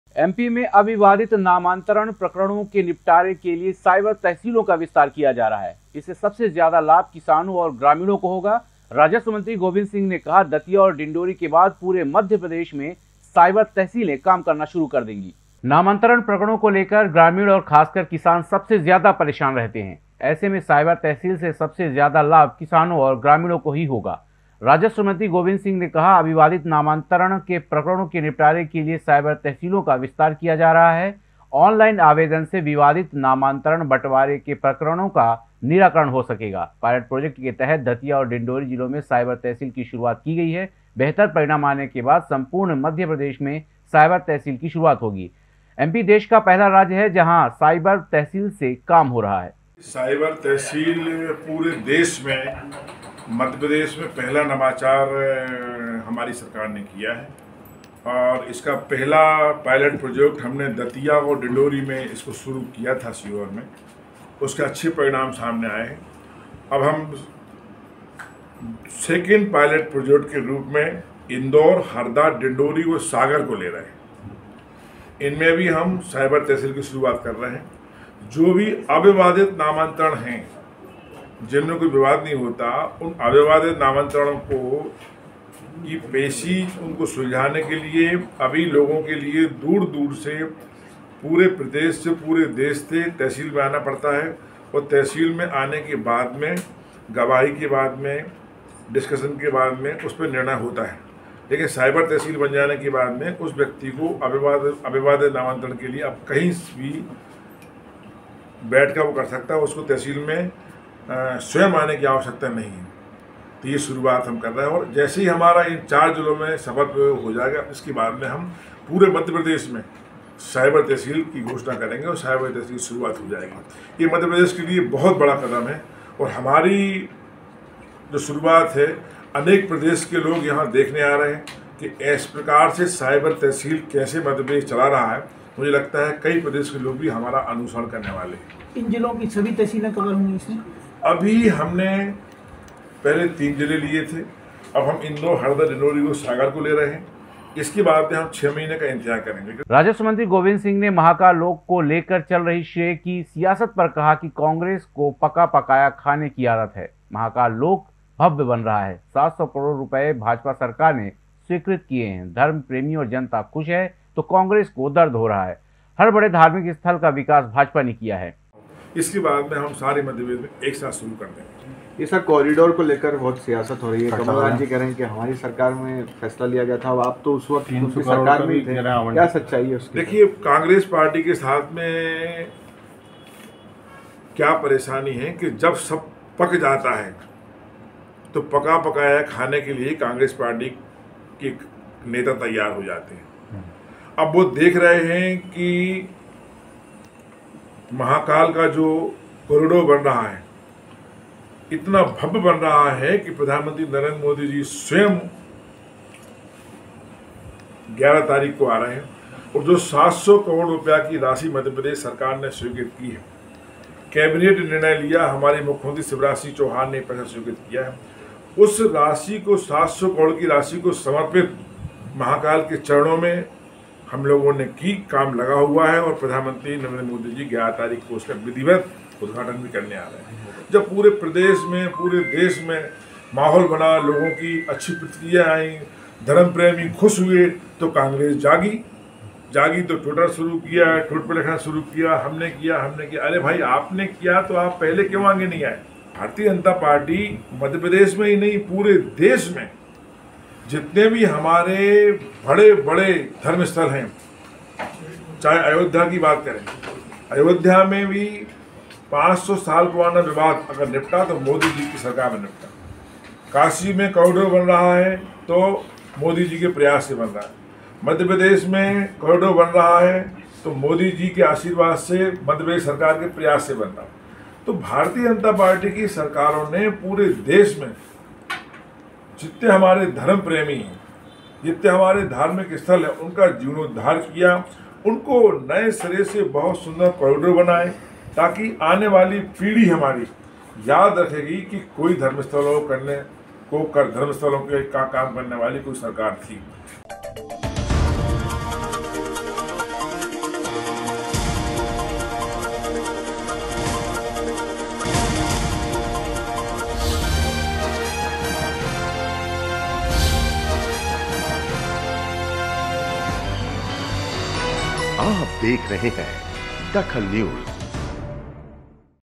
DAKHAL NEWS किसानों के काम आएगी साइबर तहसील
किसानों के काम आएगी साइबर तहसील
अविवादित नामांतरण  प्रकरण निपटेंगे